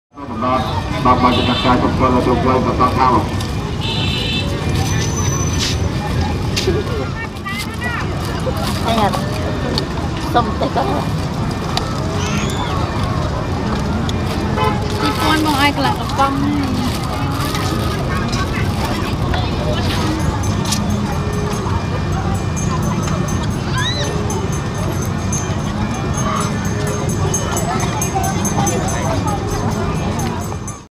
Hardworking 17 Year Old Little Lady Selling Fried Crickets
Hardworking 17 Year Old Little Lady Selling Fried Crickets - Cambodian Street Food